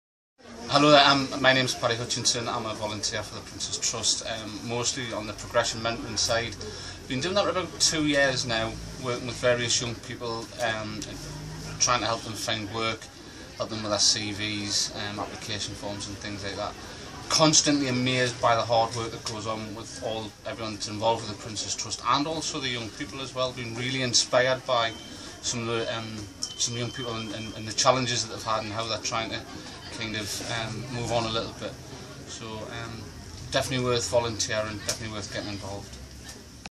Paddy, Newcastle: seeing young people overcome challenges
Paddy volunteers with The Trust as a Mentor, supporting young people who have completed one of our programmes.